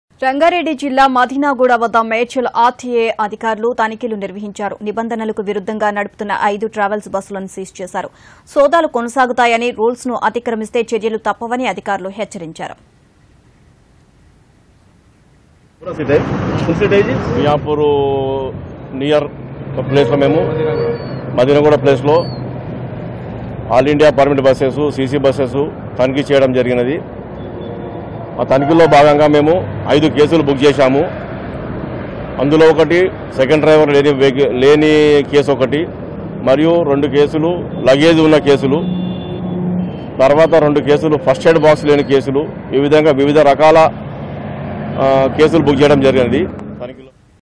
RTA seized 5 private travels busses at Madinaguda - Express TV
RTA seized 5 private travels busses at Madinaguda